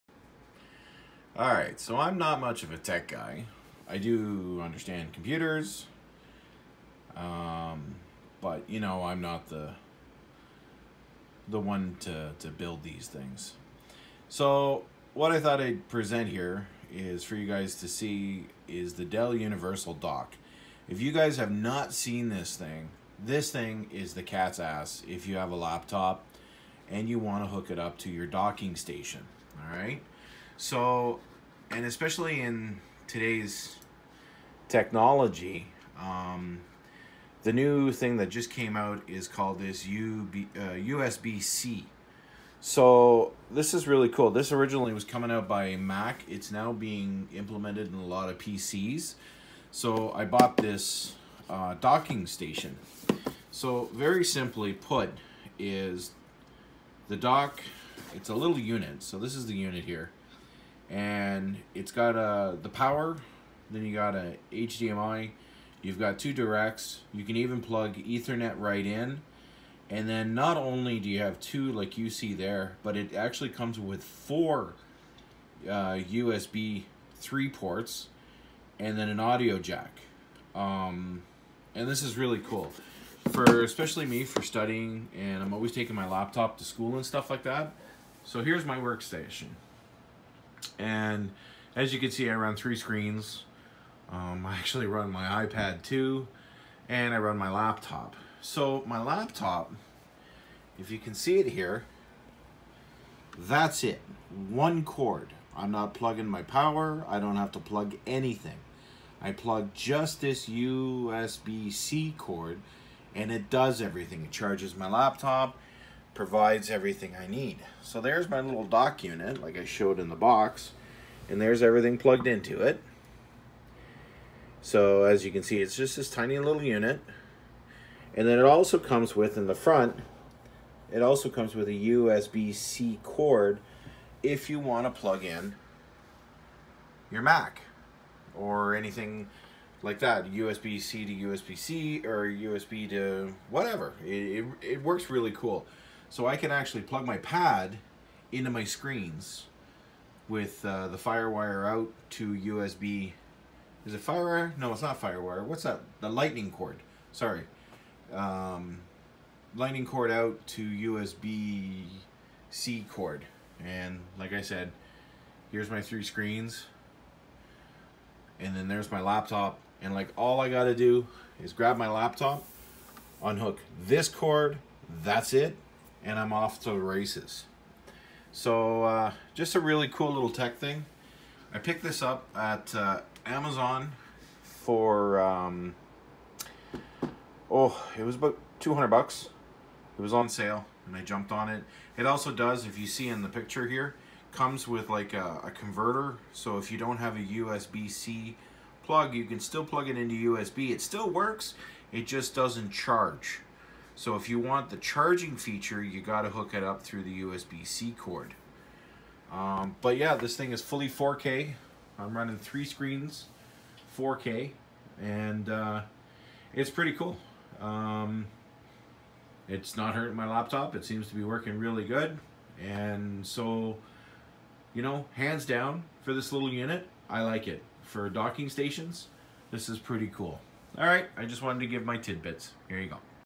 Dell D6000 Docking Station USB-C
This is my Review regarding the Dell D6000 Docking Station USB-C